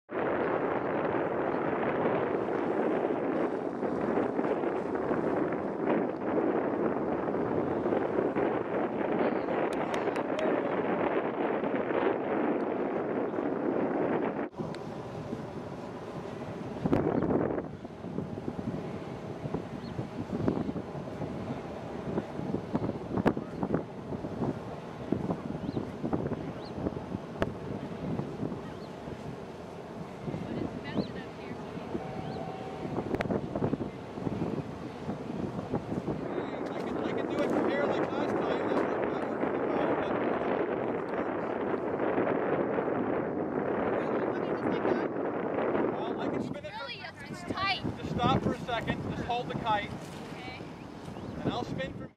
Flying the Kite
Womens Health Are You Game Road Trip goes kiteboarding!